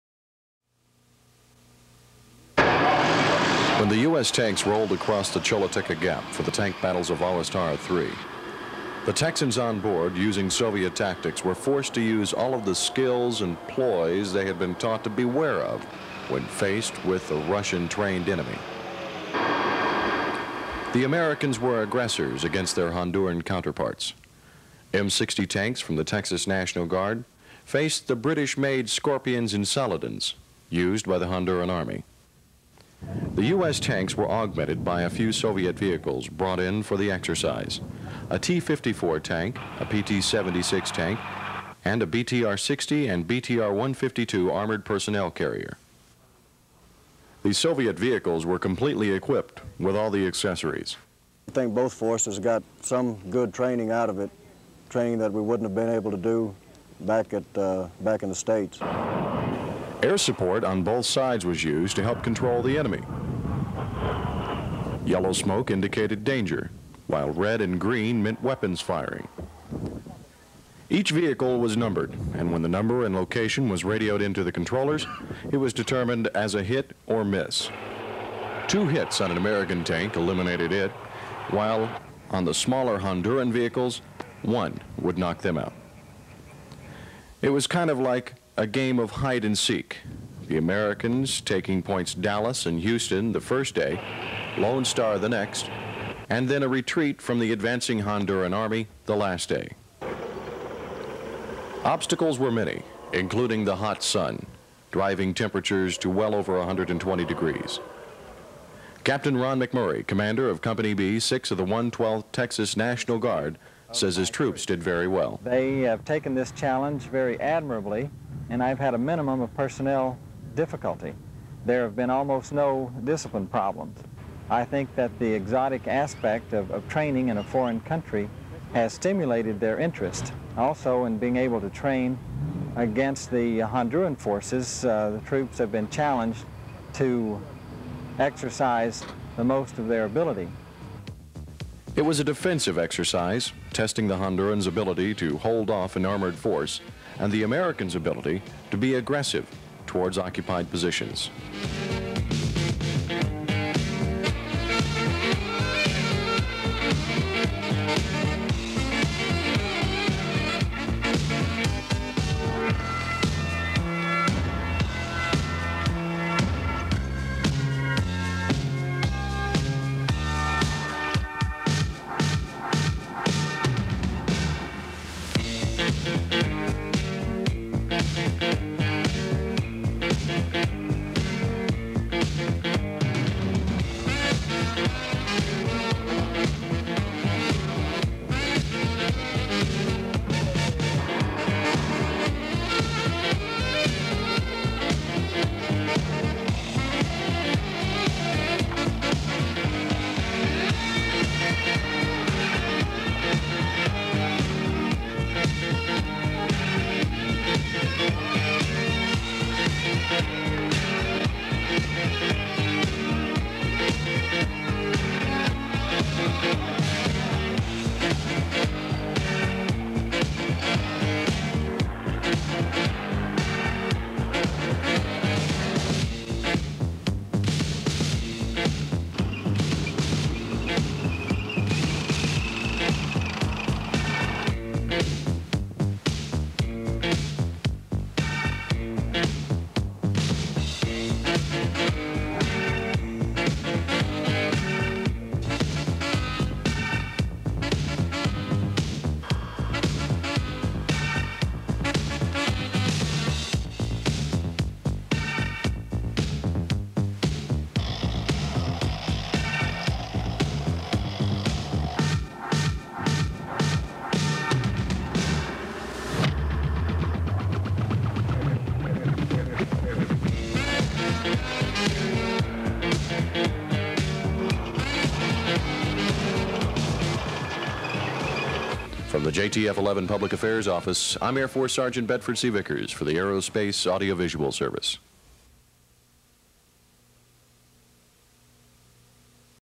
Honduras-Ahuas Tara III
Ahaus Tara III was one of two military exercises staged in Honduras during the spring of 1985.  The exercise, also known as Big Pine III, was partly military training, partly nation building to help Honduras, and partly an effort to poke the Nicaraguan Sandinista government in the eye.  It was the first time American tanks had ever been deployed to this portion Central America.

The Texas National Guard was deployed with their armor while several U.S. governors, such as Massachusetts Governor Michael Dukakis, publicly refused to commit Guard units to the deployment.  They would exercise alongside several active duty U.S. military units.

As 1st Lieutenant Kevin J. Liliy wrote in the Texas Guard newsletter of the deployment, “As news of our mission reached the press, the move was engulfed in media exposure. The political atmosphere of the day centered around President Reagan's initial Contra aid package.”

“A common thought was that while the Reagan Administration supported a U.S. show of force in the region, the implications of sending active duty units seemed politically unsound.”

The troop carriers staged war games within three miles of the Nicaraguan border near San Lorenzo in with M-60 tanks and armored personnel carriers rumbled over hot, dry scrub land in a mock attack on Honduran troops in the Choluteca Gap.  This was surmised as the likely place the Nicaraguan tanks might try to invade Honduras.  Nicaraguan troops were already spotted chasing Nicaraguan rebels, known as Contras, across the Honduran border as they tried to escape.  Several Contra camps were reportedly positioned just inside the Honduran border.

The exercise kicked off less than a week after President Reagan called for peace talks between the Sandinistas and the U.S.-backed Contra rebels. Nicaraguan officials have rejected the proposal.

Meantime, the U.S. troops in the Choluteca Gap reported it was not unusual for them to suddenly see armed Nicaraguan troops on foot just yards away from them, watching the military buildup and activities in Honduras.

(This is an upload of a historical production.  Copyright permissions were gained at the time of production.)